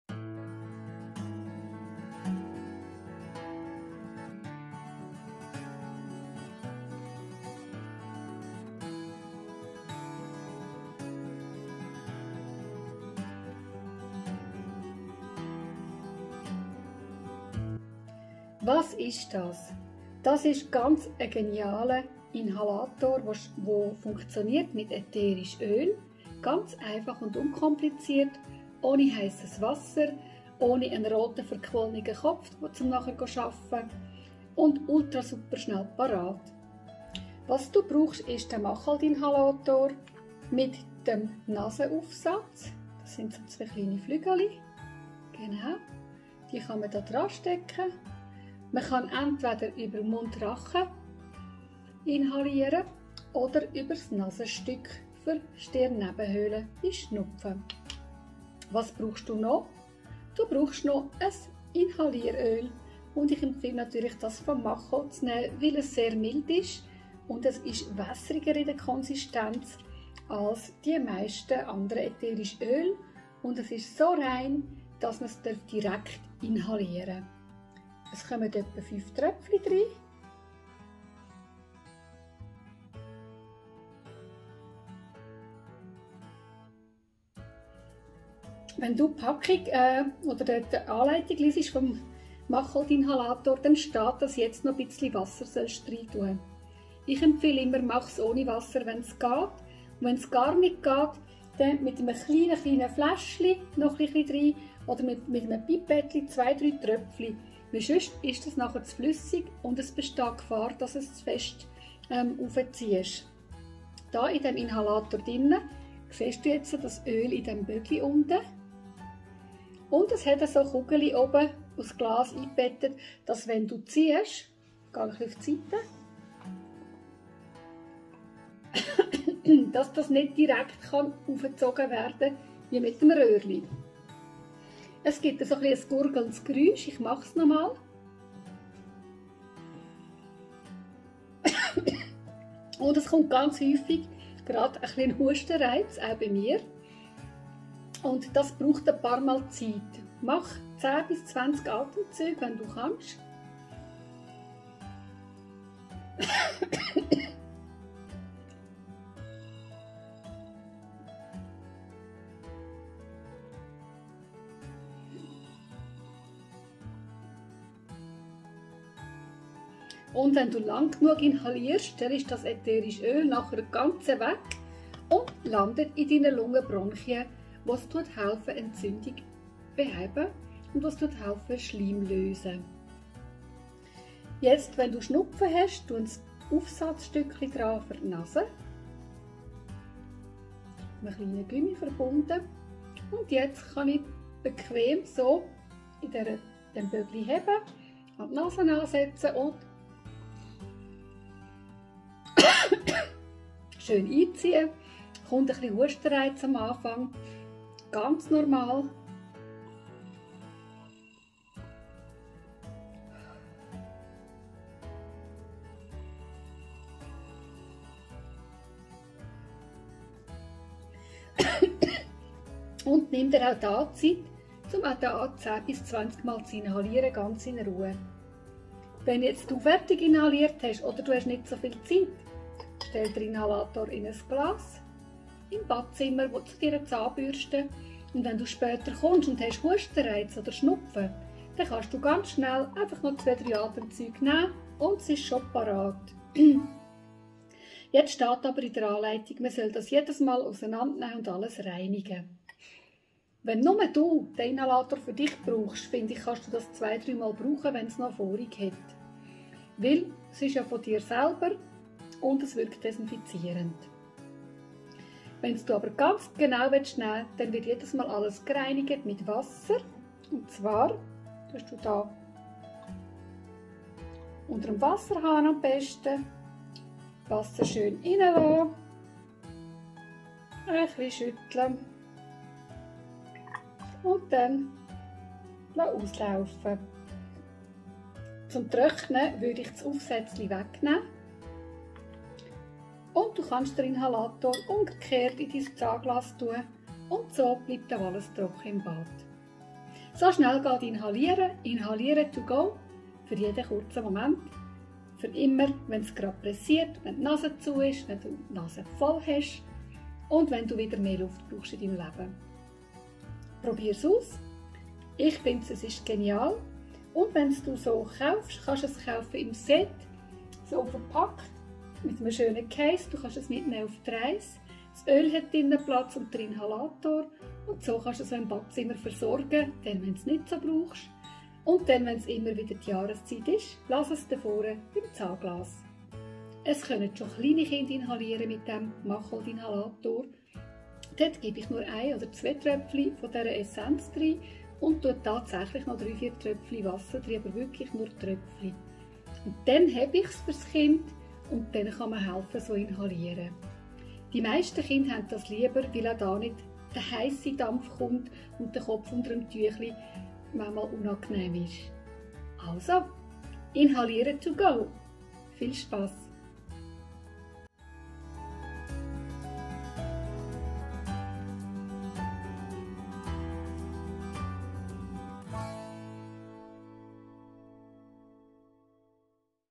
Inhaliere mit ätherische Öl - Mundart
Nasepfleg isch wichtig bi Heuschnupfe, Inhalationsallergie u wäge Infektione! So gaht inhaliere mit ätherische Öl